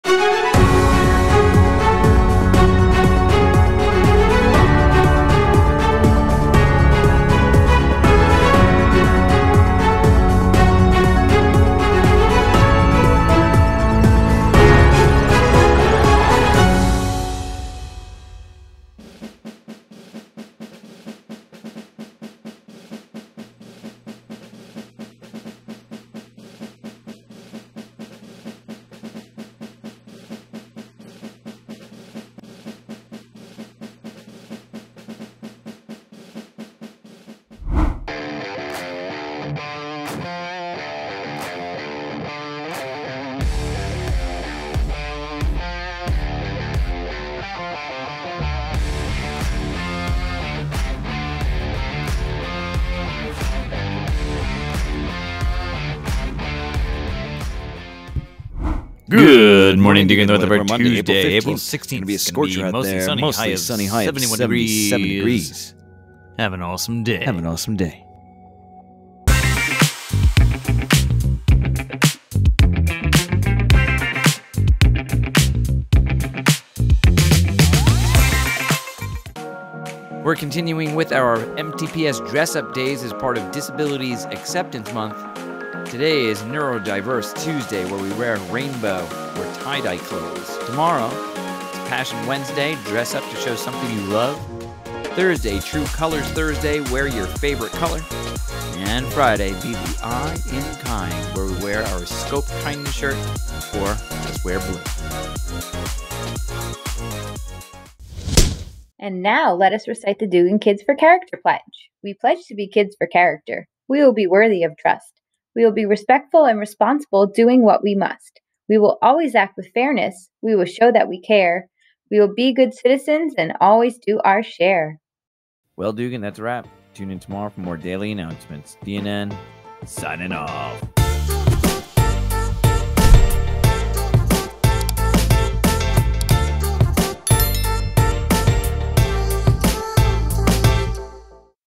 DNN S3 Ep128: Neurodiverse Tuesday
Today is Neurodiverse Tuesday as we continue to celebrate Disabilities Acceptance Month.